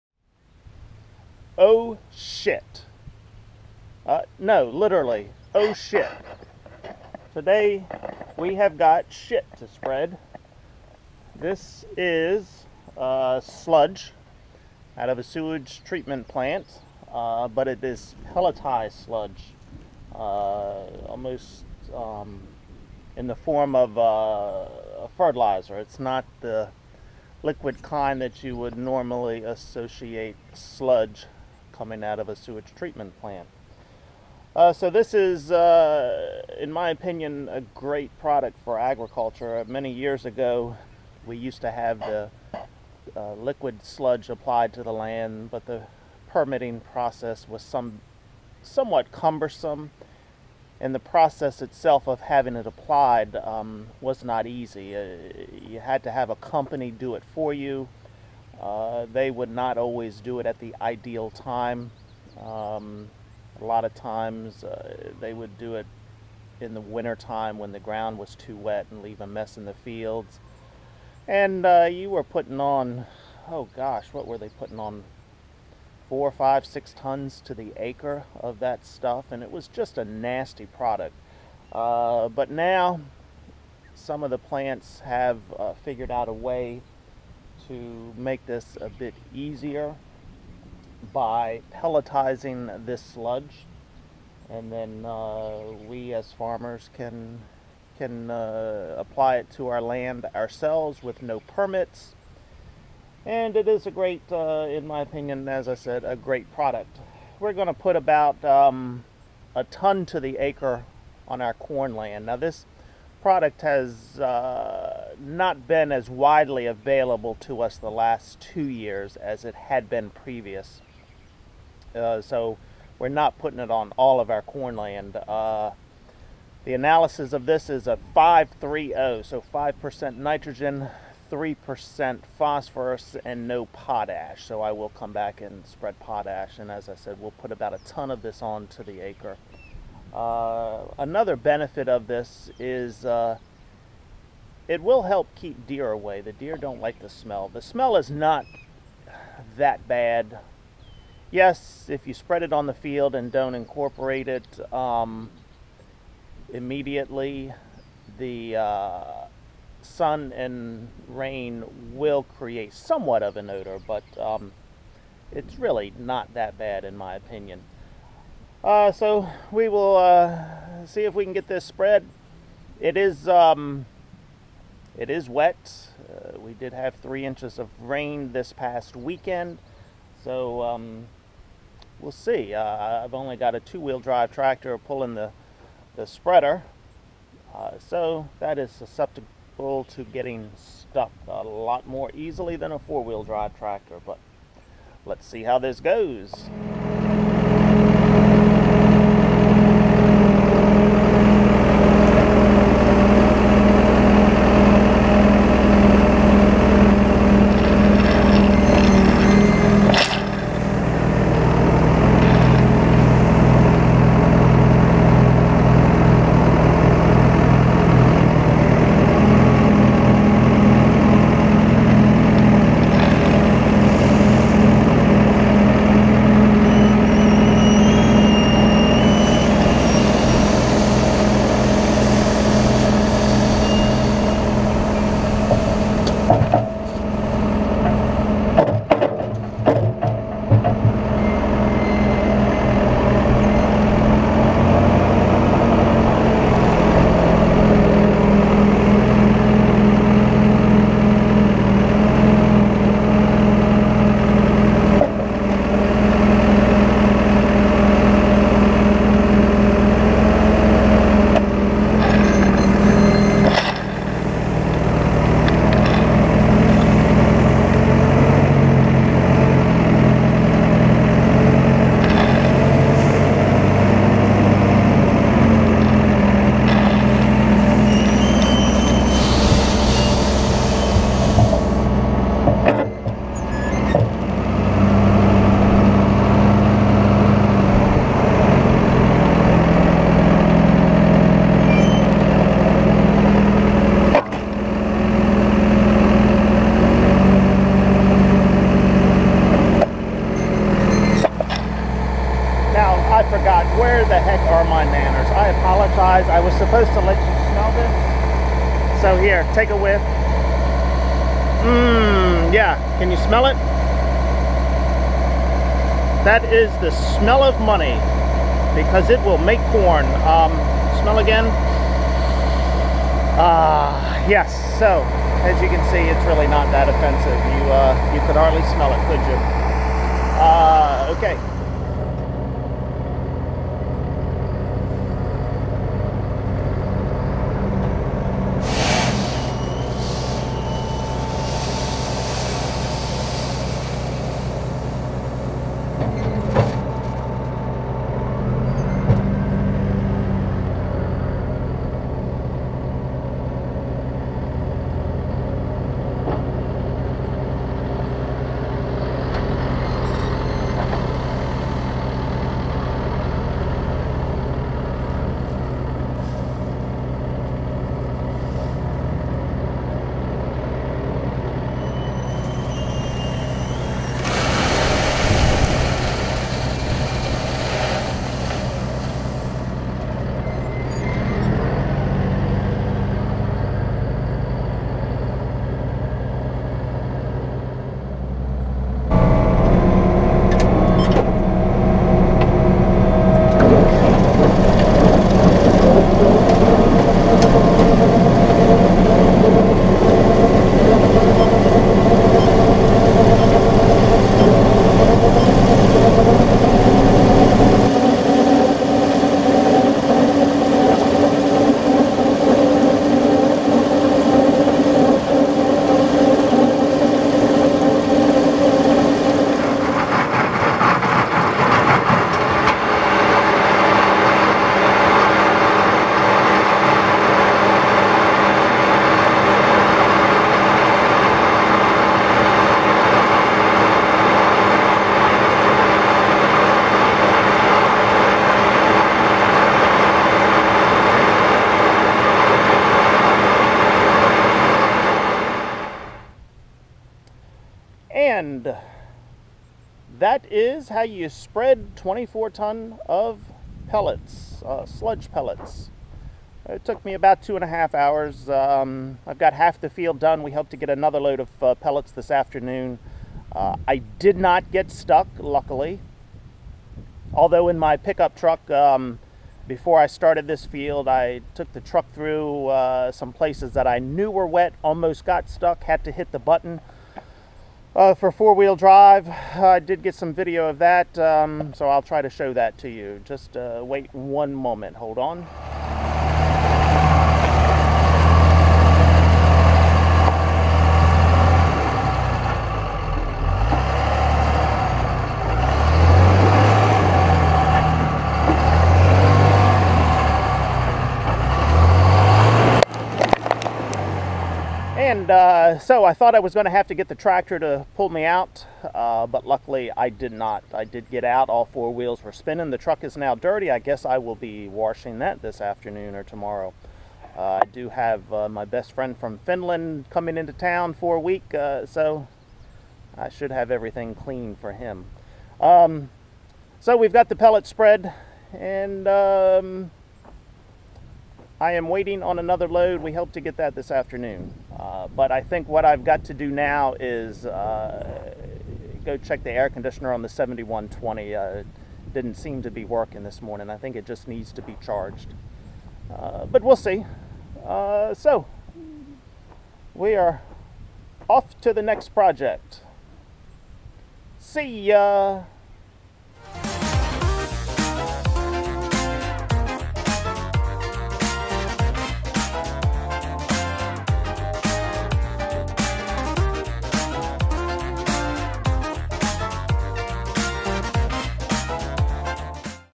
Spreading Sludge Pellets on Corn Fields
May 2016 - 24ton of Sludge Pellets (5-3-0) being spread on corn fields in Southern Maryland.